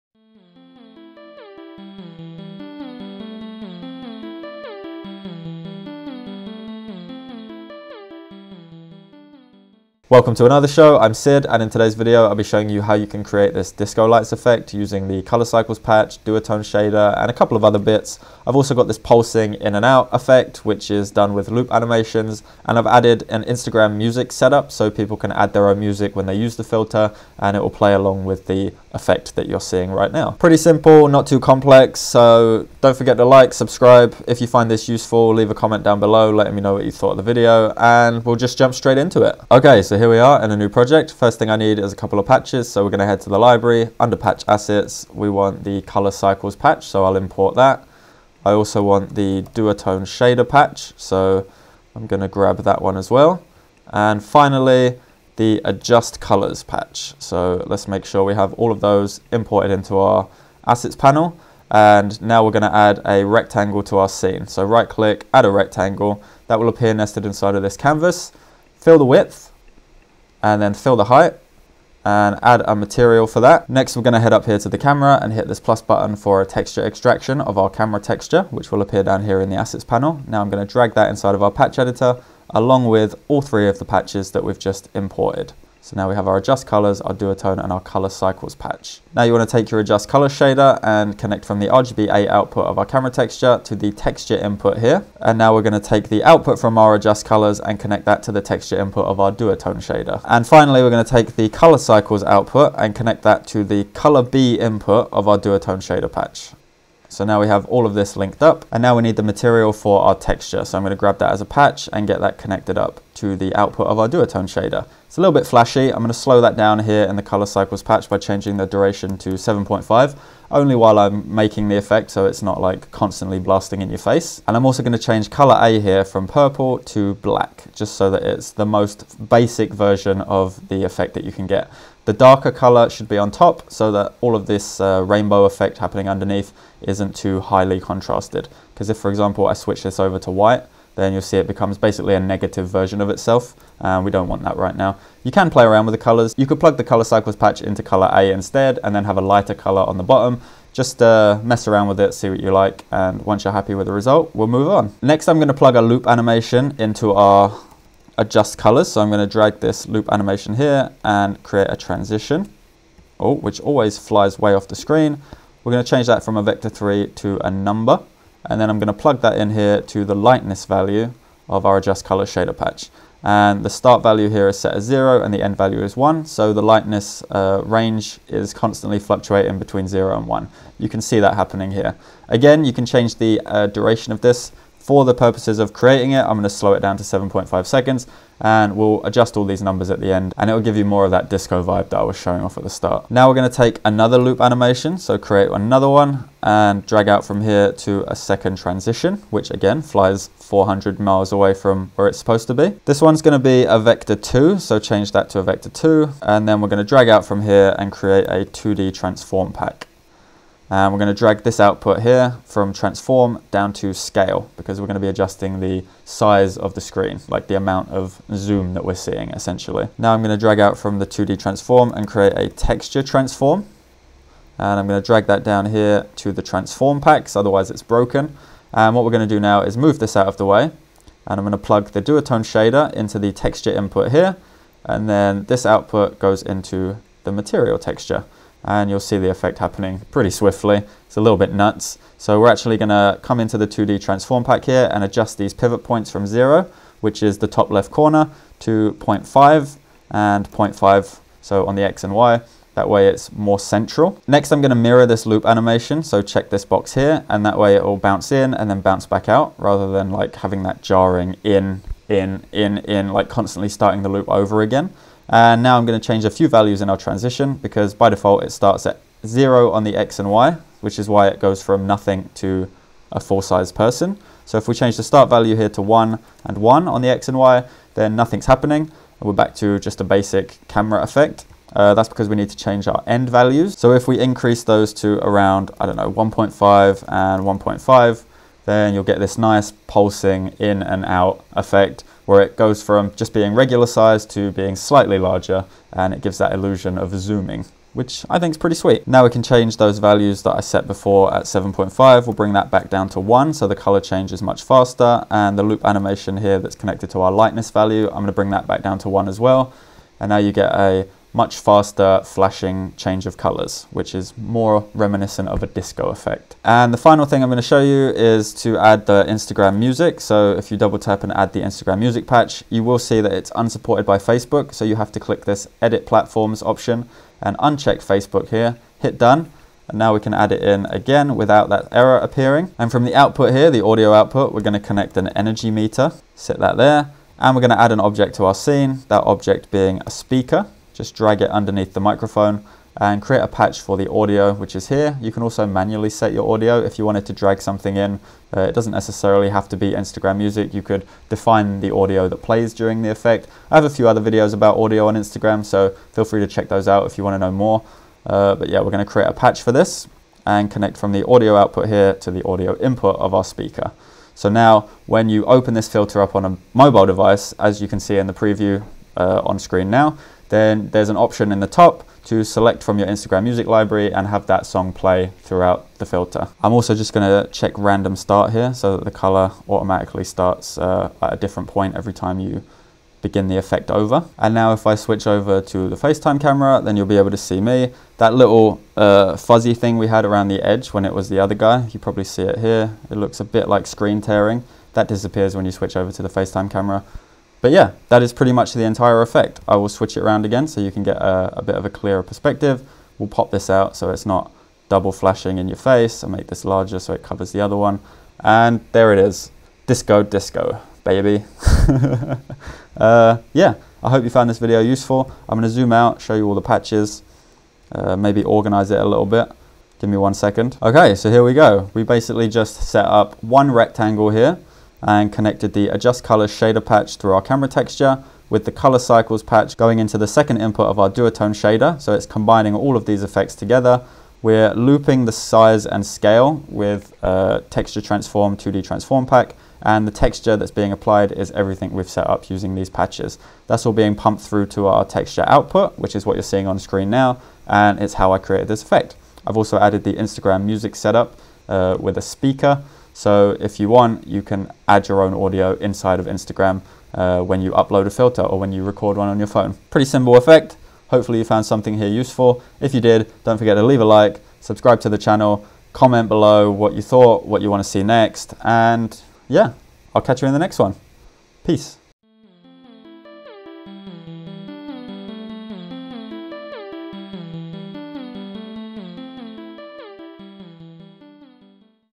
Disco Lights (Color Cycle, Duotone, IG Music) | Instagram/Facebook | Meta Spark Studio | AR Tutorial
- - Assets - -

In this video I demonstrate how to create a disco light effect using the colour cycle, duotone and adjust colours patches with texture transform for zoom and Instagram music library features for filters on Instagram & Facebook using Spark AR Studio on MacBook Pro 2020.